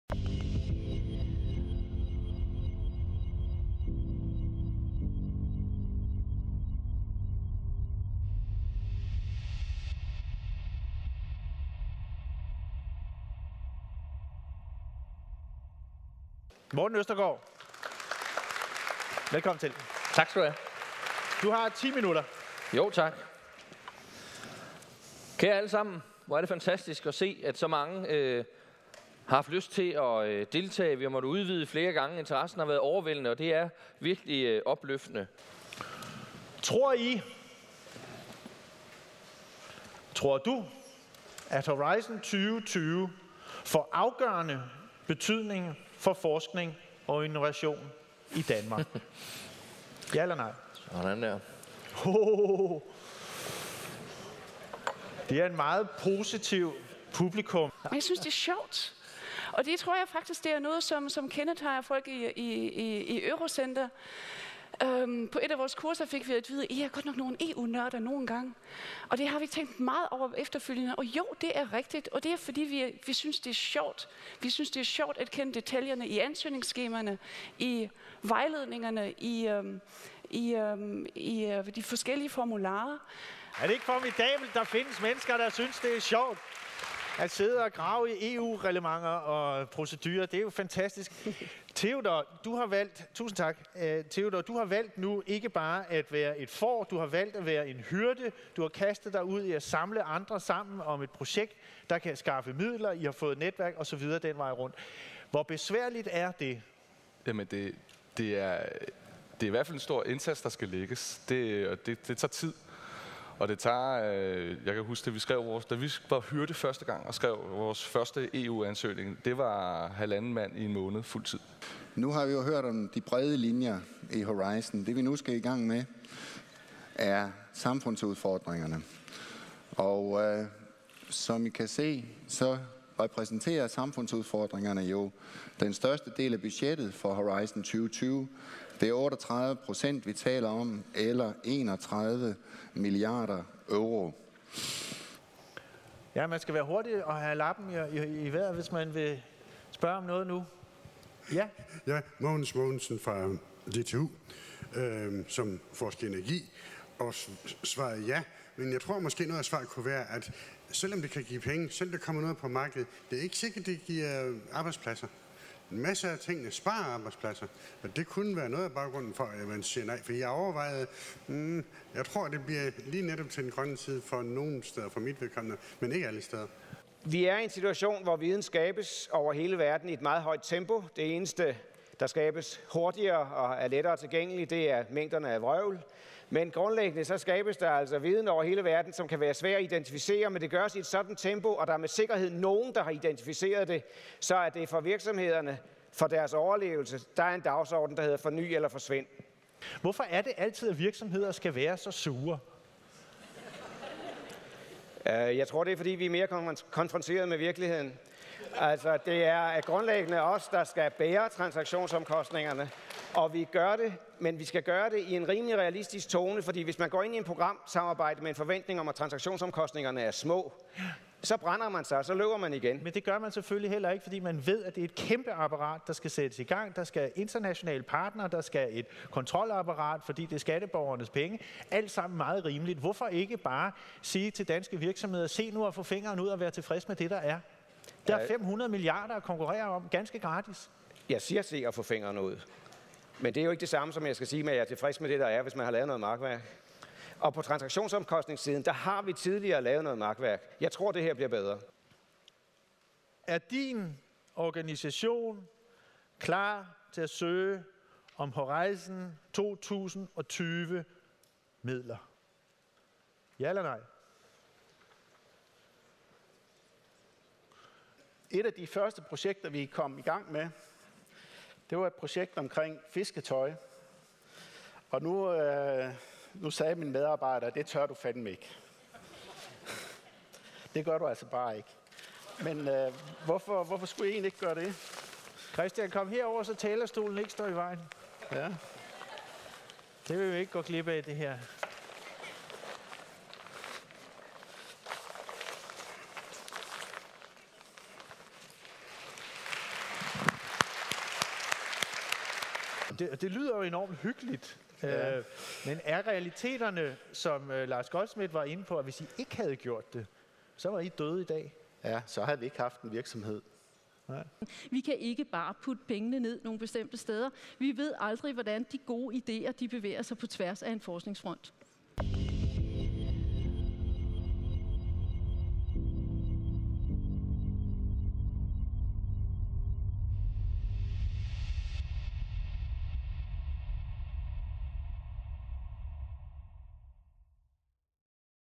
Konferencen "Nutidens udfordringer - fremtidens løsninger" om Horizon 2020
Sammenklip fra konferencen "Nutidens udfordringer - fremtidens løsninger" om Horizon 2020 den 29. oktober 2013 i Tivoli Congress Center. På konferencen lancerede Uddannelsesministeriet EU's nye forsknings- og innovationsprogram, Horizon 2020. Konferencen tiltrak ca. 1.600 deltagere fra universitetsverdenen og erhvervslivet.